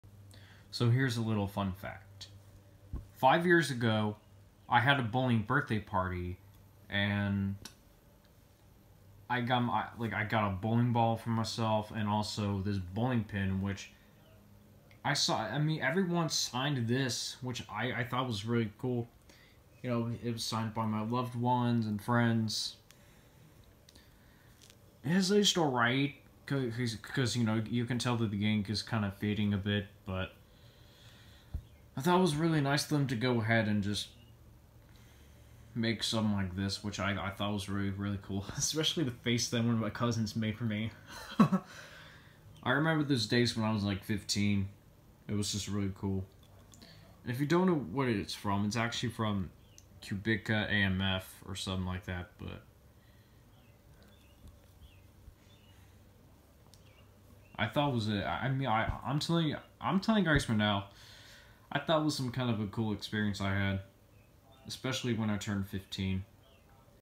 My Bowling Pin From My 15th Birthday 🎳
Almost five years ago, I had a birthday party at the bowling alley. Not only that I got my own bowling ball, but I also got a bowling pin. Every loved one and friend of mine signed this pin, and I was impressed by everyone who supported me during that time. Every birthday of mine is very unforgettable.